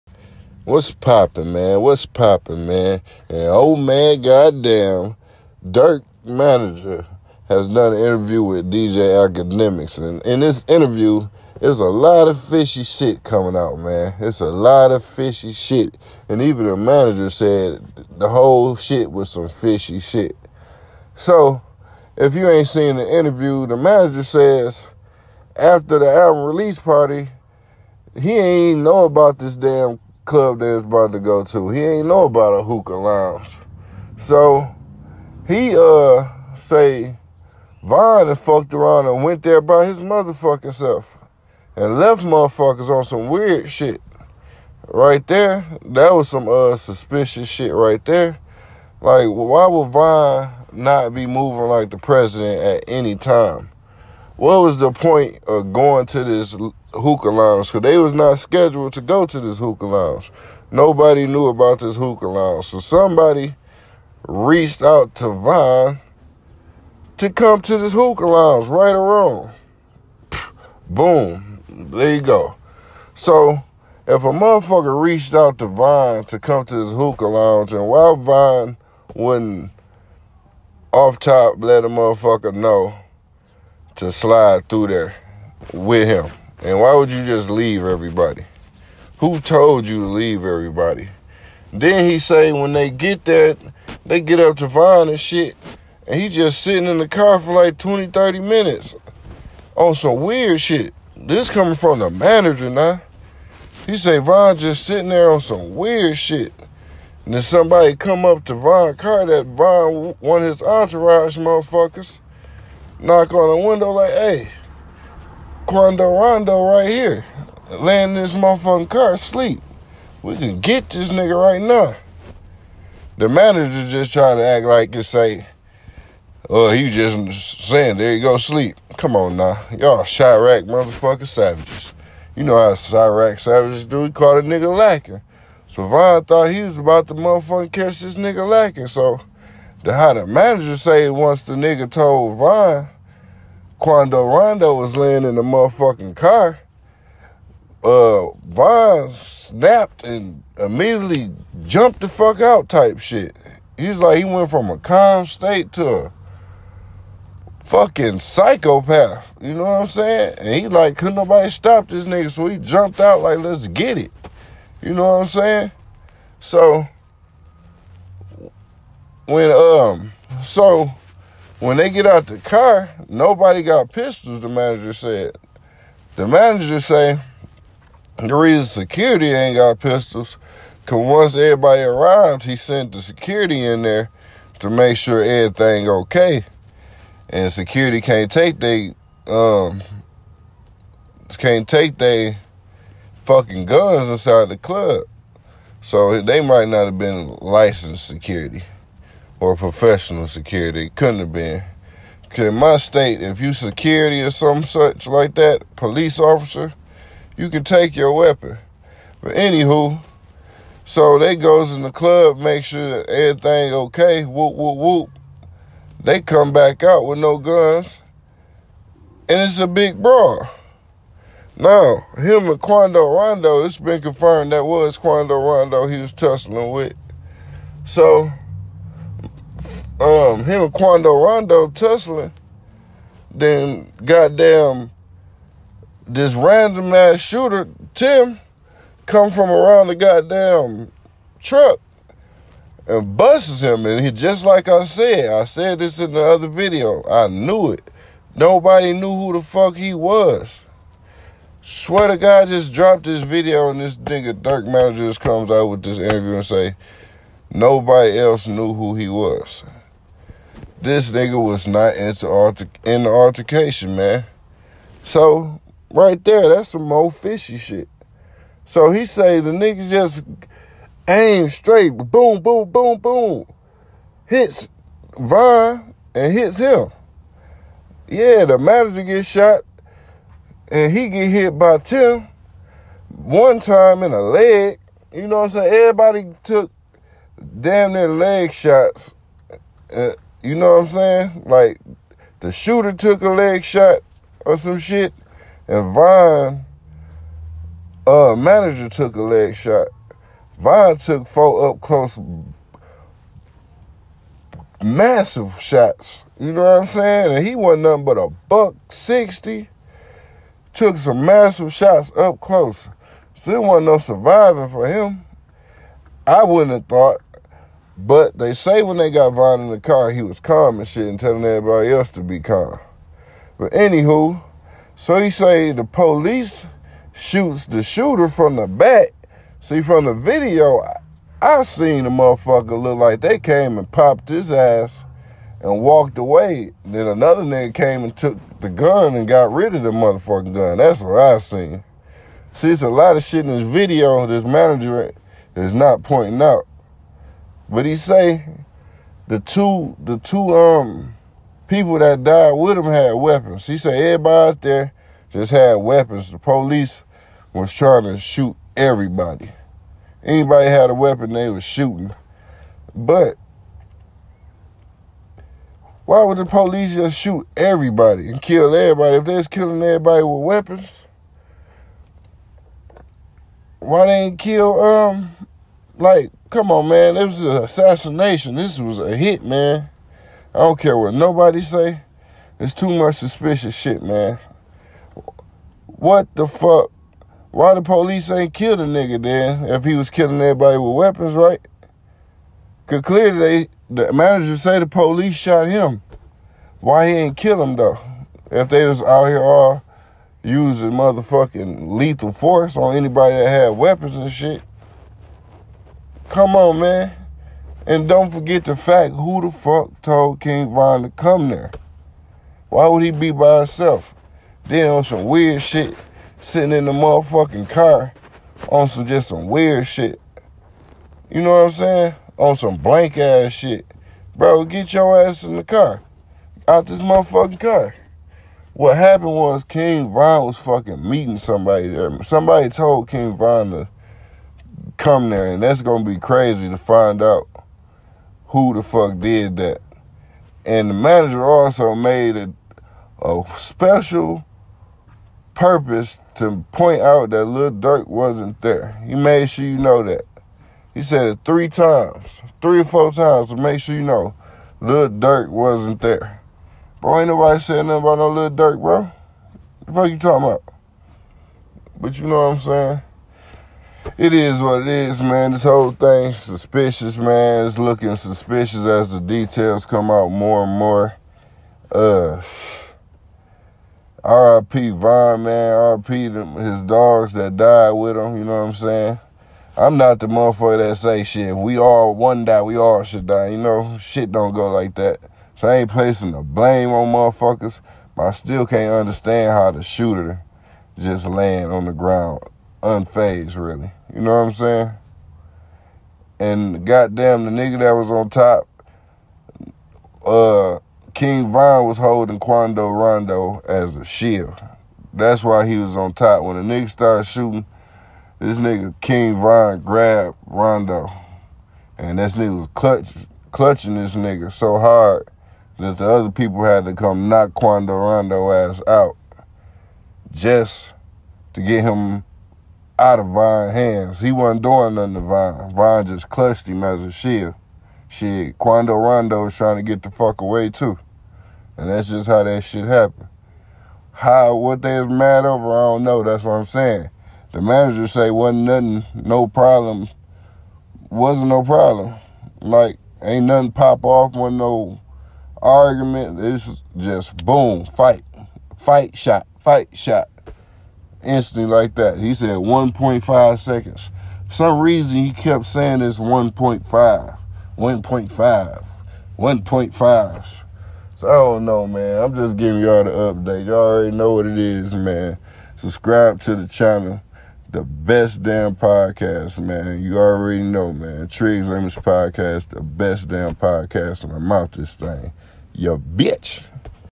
King Von manger says he doesn’t know why king Von went to the club or who invited him.”it was weird”
King vons Manager recaps night of the death of King Von and the fight between King Von vs Quando rondo